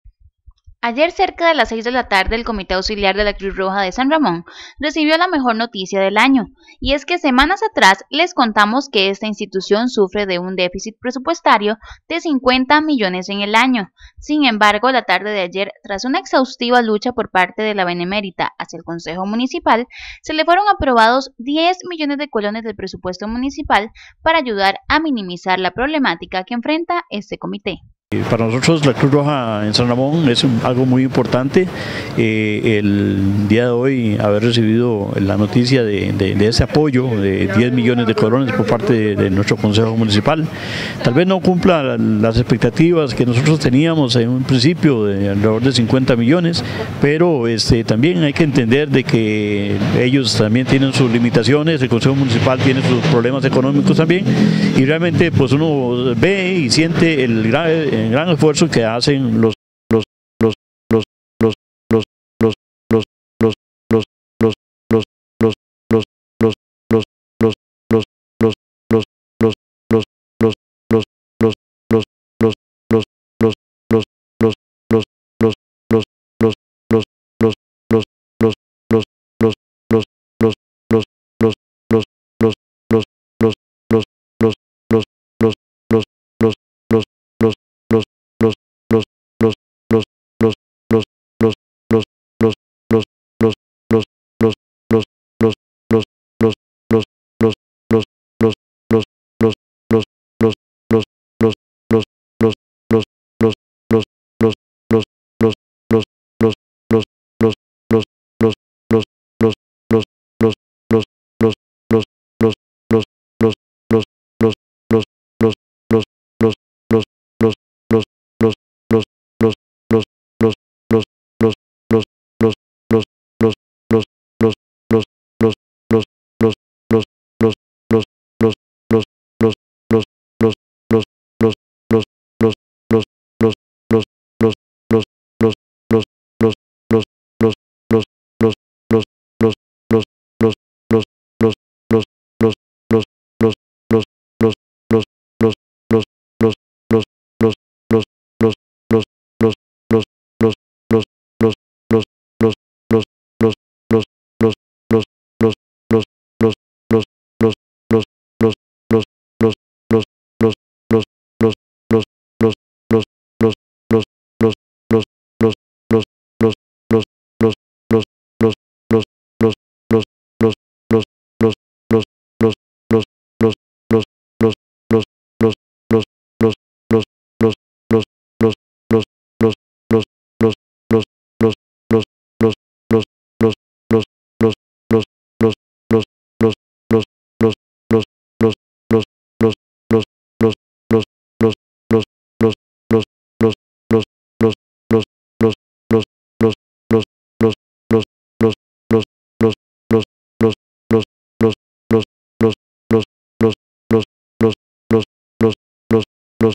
Concejo Municipal aprueba ¢10 millones a Cruz Roja ramonense
En sesión municipal, la Municipalidad de San Ramón aprueba darle 10 millones de colones a la Cruz Roja del cantón para minimizar el déficit económico que enfrentan, este monto será entregado en dos tractos durante el año.